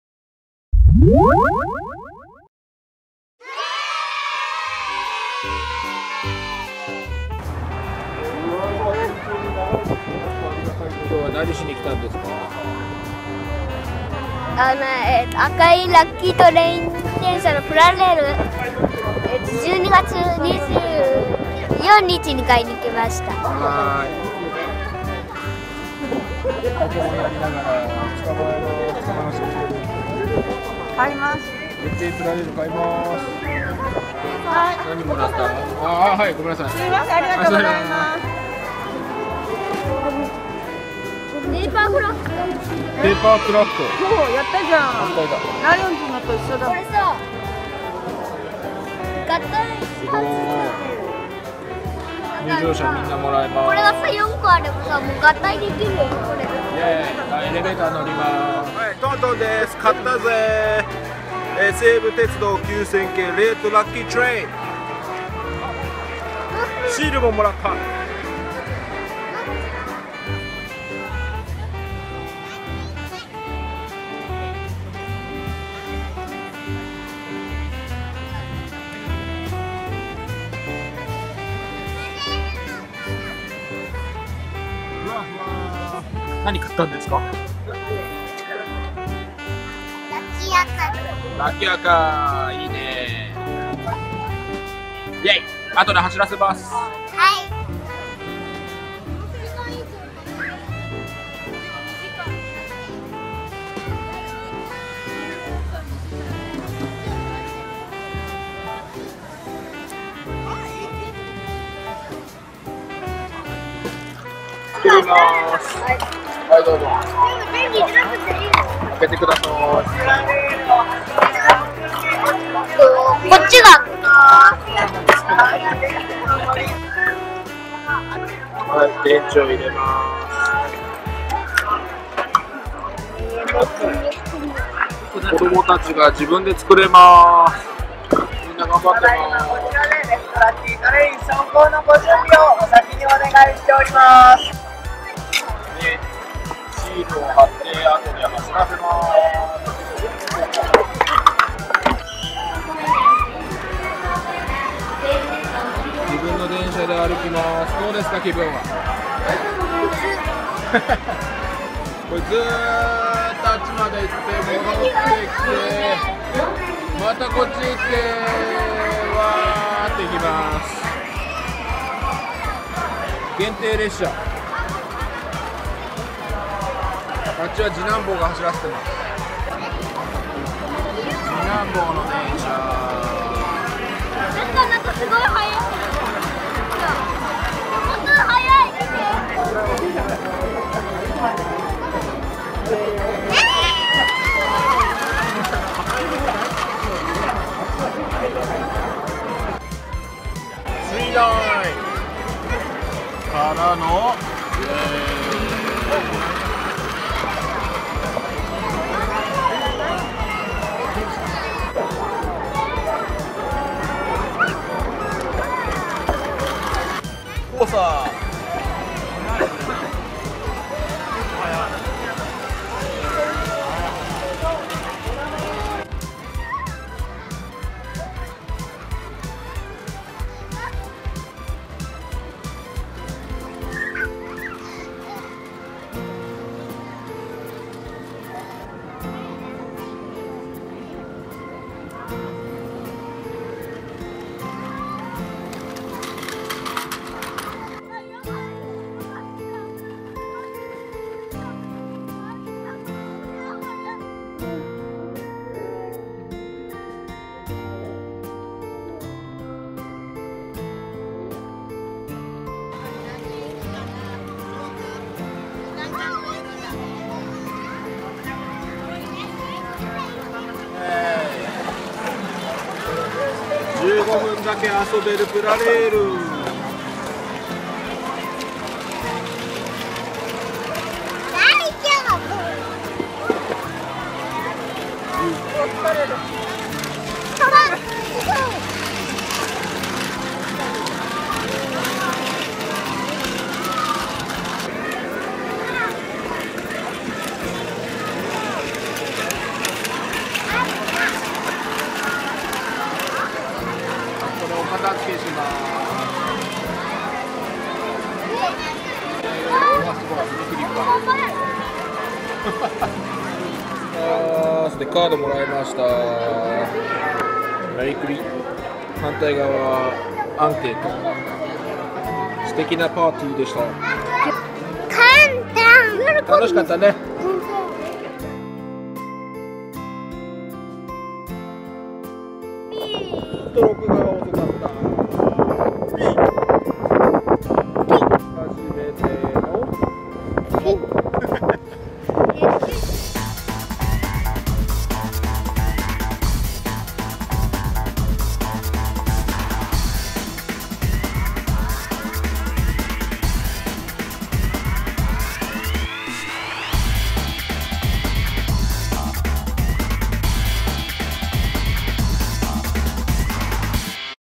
【プラレール】西武鉄道のレッドプラレールフェスティバルに行ってきた！限定ラッキートレインをゲット！ Plarail event! Seibu, limited RED HAPPY TRAIN!!
ハッピートレインの詳細は下記を参考にしてください。

見たら幸せが訪れるかも!?『幸運の赤い電車』とは

また鈴川絢子さんのチャンネルでも同イベントの様子が紹介されています！(トントンも足元がチラリと！)

仮面ライダーゴースト　レジェンドゴーストアイコン